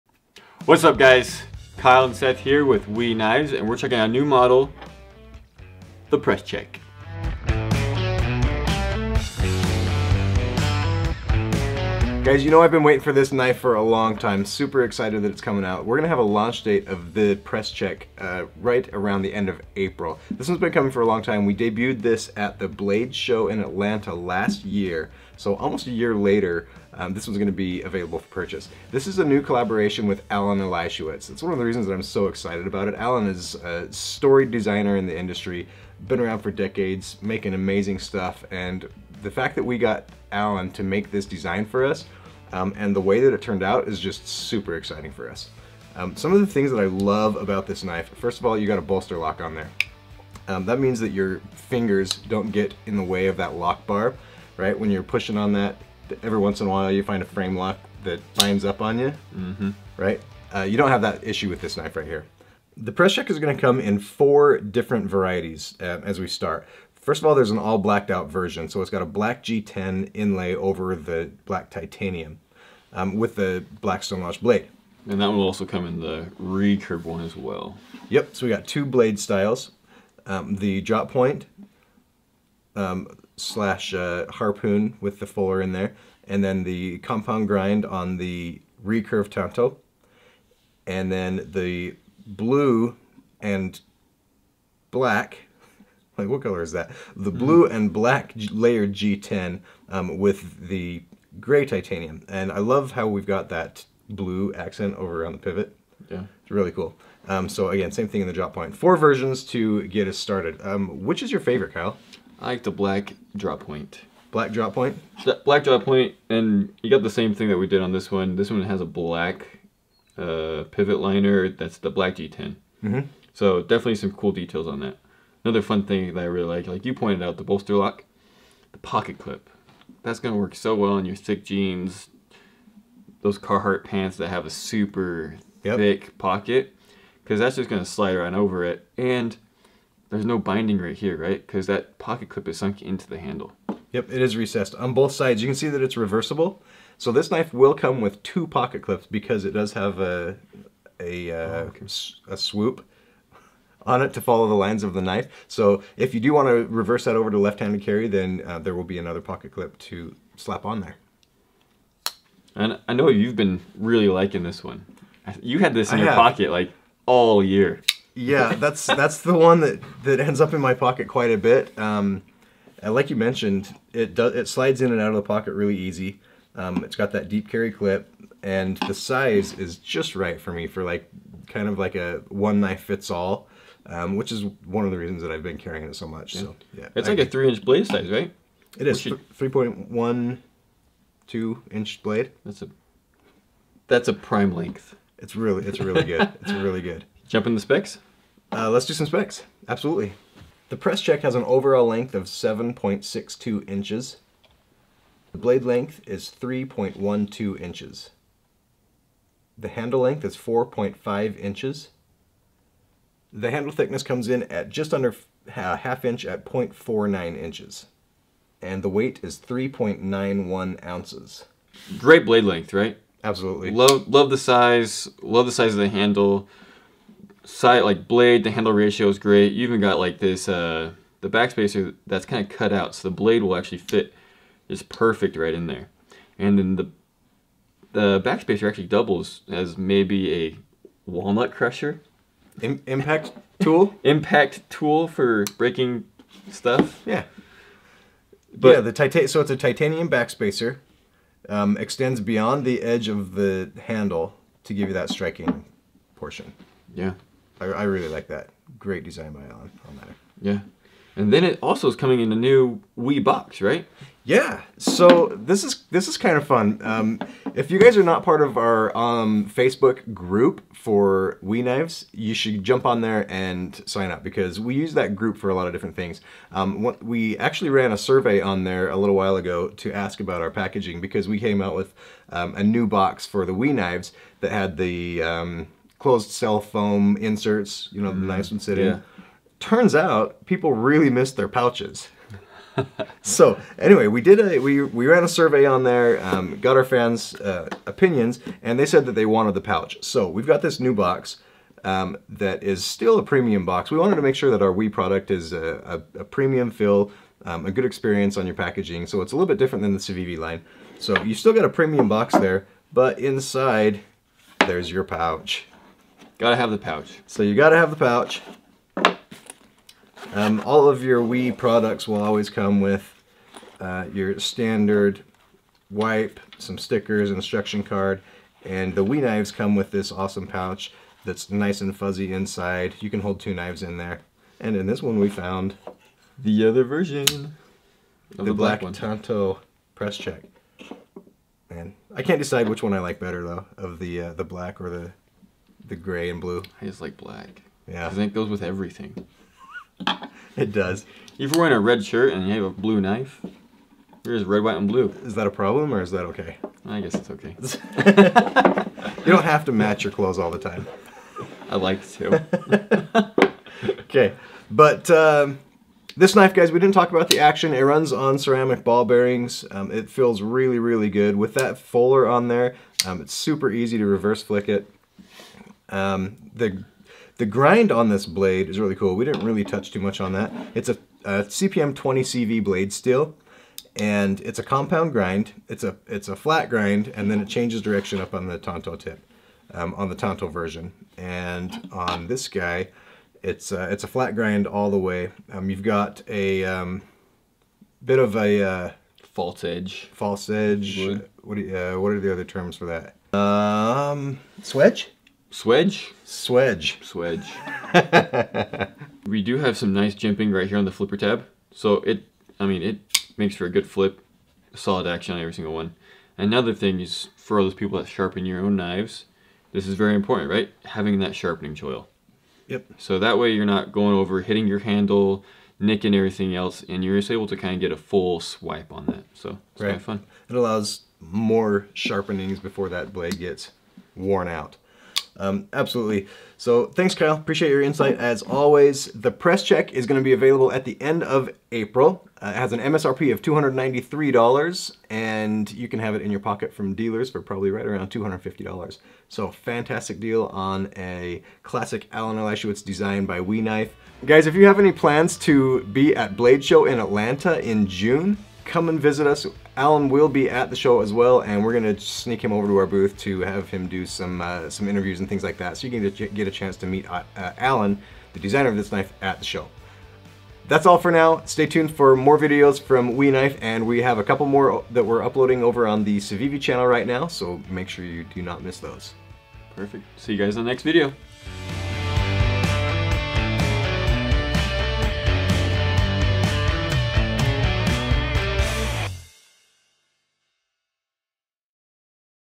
The WE Press Check designed by Allen Elishewitz is available NOW!!
Take a quick look at the new Allen Elishewitz design from WE Knife, the Press Check.